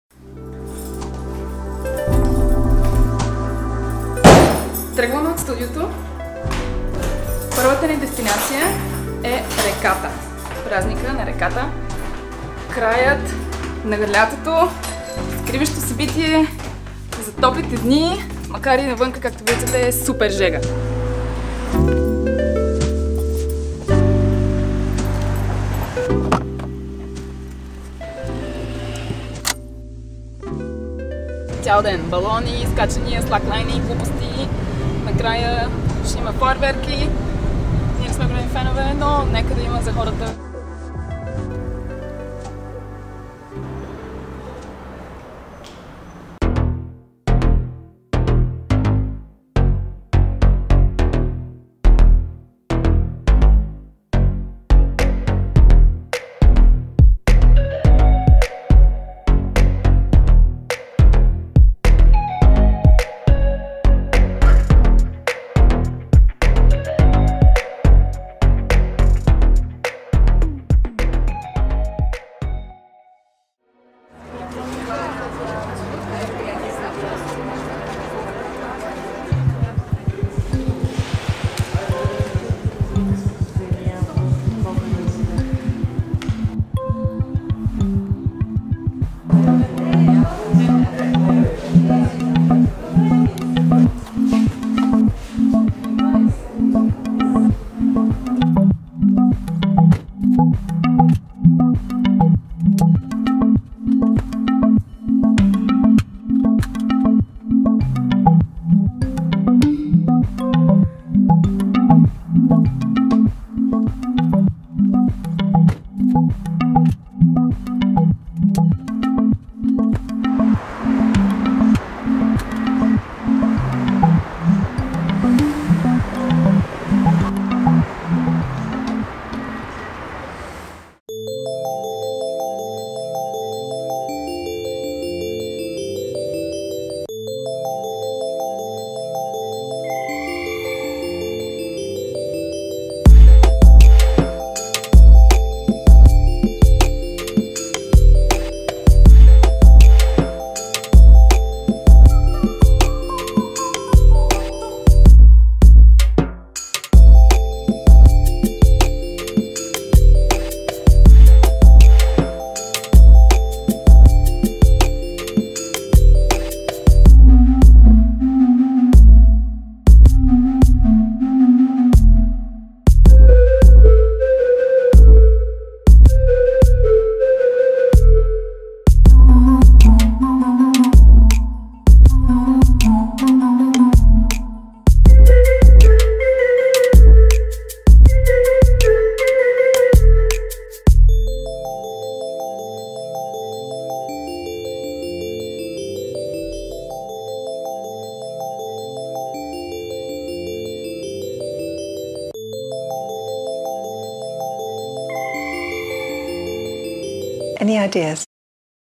September celebration with Plovdiv 2019 and the Contemporary Art Week in the Ancient Bath, Plovdiv
Прекарахме 1-ви септември на Празника на реката, организиран от Пловдив 2019, а после се насладихме на откриването на Седмицата на съвременното изкуство, в Баня Старинна.

We spent a few hours exploring the River Feast event organized by 2019 and then we immersed in the Contemporary Art Week in the Ancient Bath, in Plovdiv.